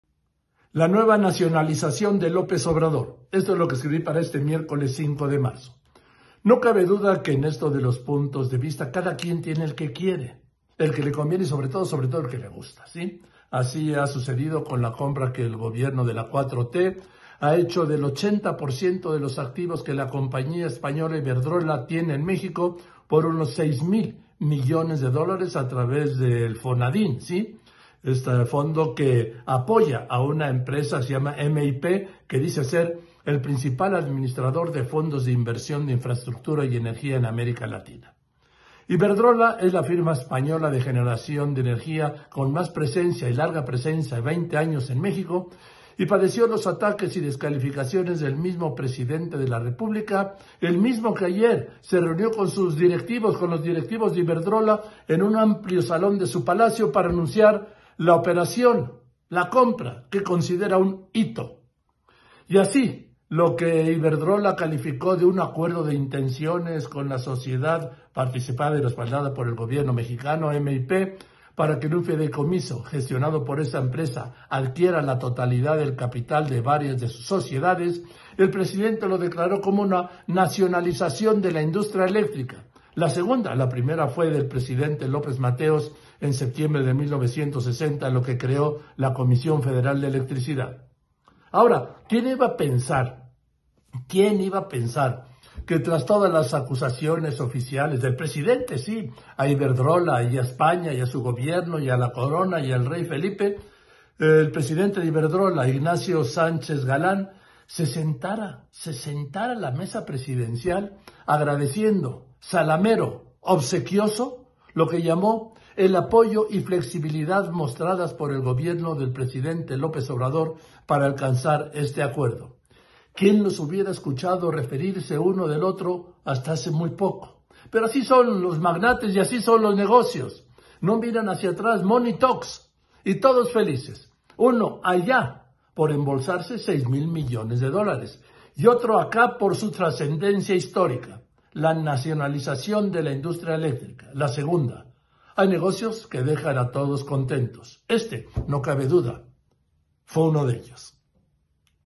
La nueva nacionalización
Pero así son los magnates y así son los negocios. No miran hacia atrás, money talks, y todos felices. Uno, allá, por embolsarse seis mil millones de dólares, y otro, acá, por su trascendencia histórica: la nacionalización